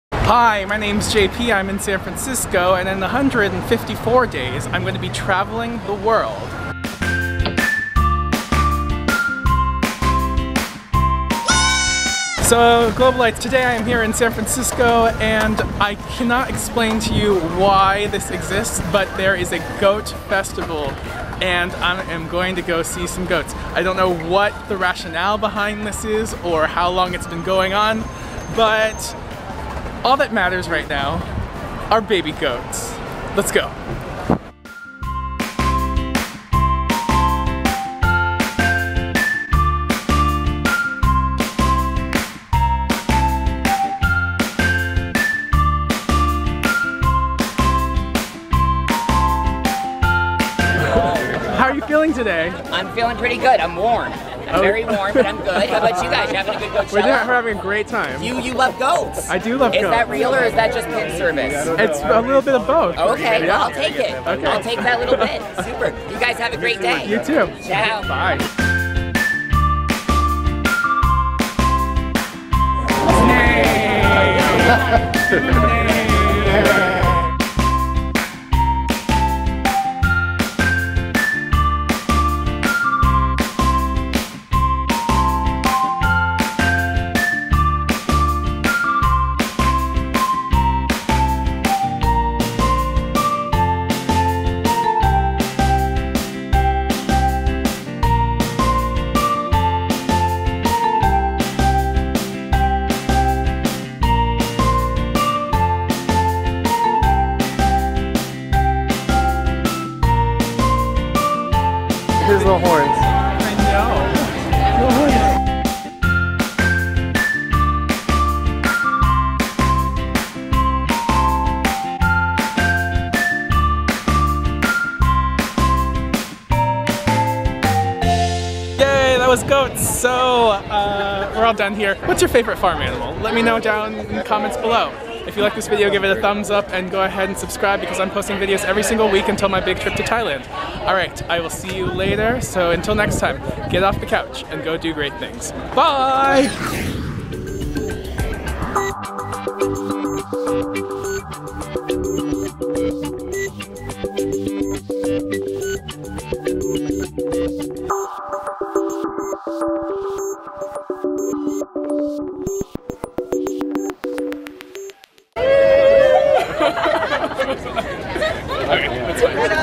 Baby Goat Festival! \\ Excursion Vlog
Baby goats!!! AKA Goatchella, CUESA's seventh annual Goat Festival had over 15,000 people RSVP on Facebook! Luckily, we got there just in time to see these little cuties before the festival closed shop. Nnnaaayyyy!